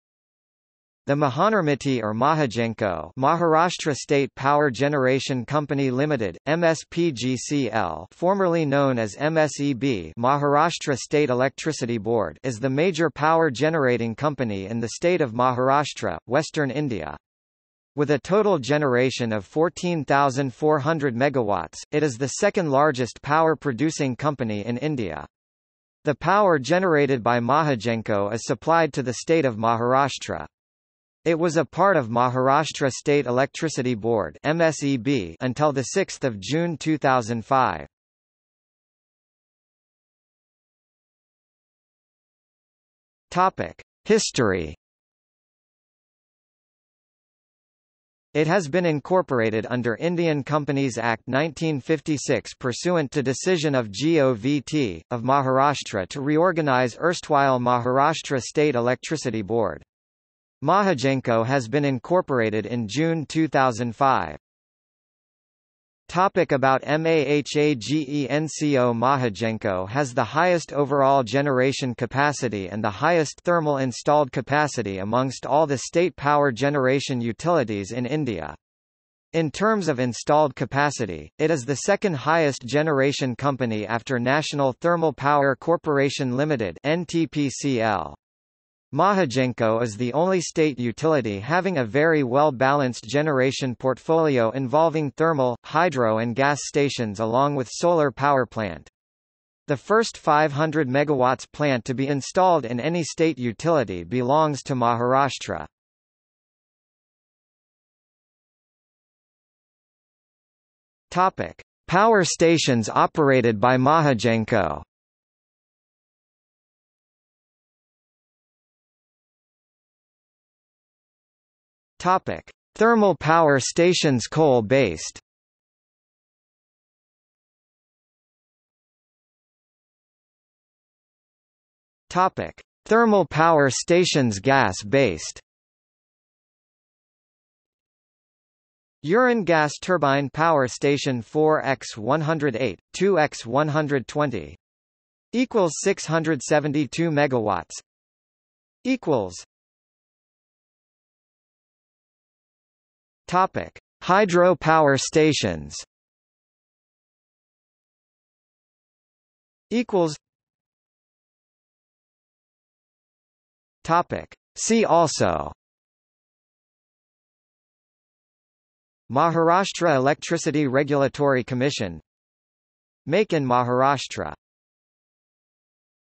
Maharashtra State Power Generation Company Limited | Wikipedia audio article
00:00:39 1 History
00:01:05 2 About MAHAGENCO
00:01:47 3 Power Stations operated by Mahagenco
00:01:58 3.1 Thermal Power Stations Coal based
00:02:08 3.2 Thermal Power Stations Gas based
00:02:25 3.3 Hydro Power Stations
00:02:29 4 See also
00:02:39 5 References

- Socrates

SUMMARY
The Mahanirmiti or Mahagenco (Maharashtra State Power Generation Company Limited - MSPGCL) formerly known as MSEB (Maharashtra State Electricity Board) is the major power generating company in the state of Maharashtra, Western India. With a total generation of 14400 MW, it is the second largest power producing company in India. The power generated by Mahagenco is supplied to the State of Maharashtra. It was a part of Maharashtra State Electricity Board (MSEB) until 6 June 2005.